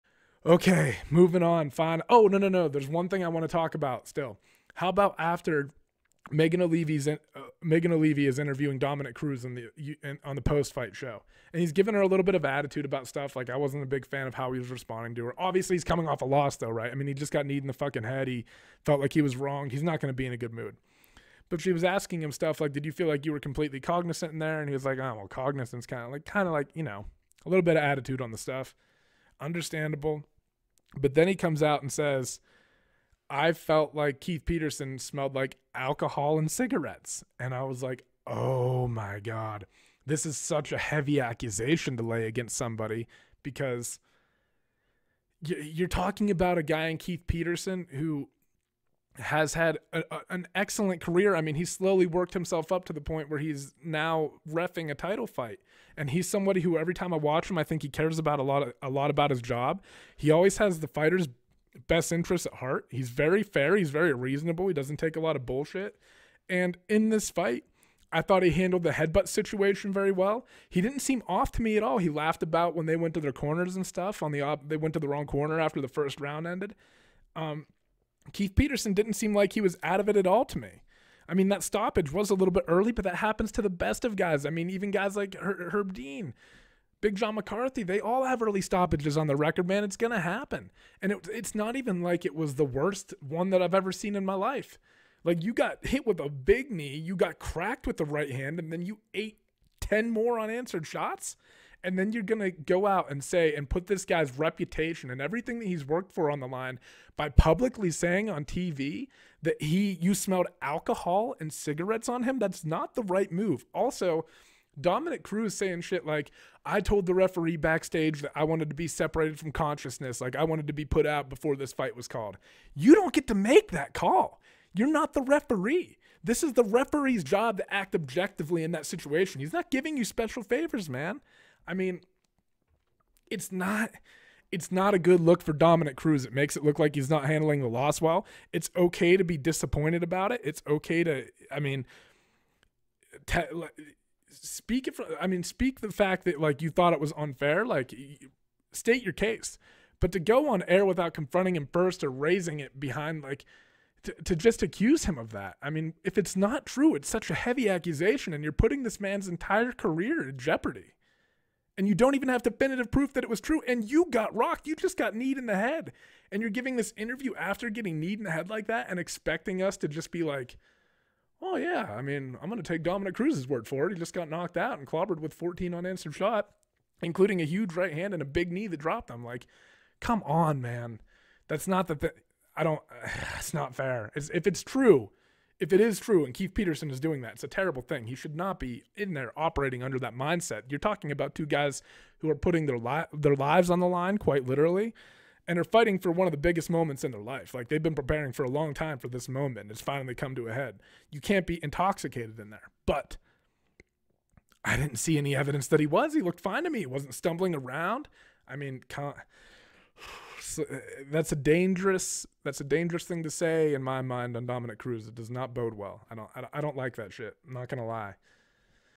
Dominick Cruz: Referee Keith Peterson 'smelled like alcohol and cigarettes'
Dominick Cruz laid a heavy accusation against one of the most professional refs in the game, Keith Peterson, after his loss Saturday night to Henry Cejudo at UFC 249.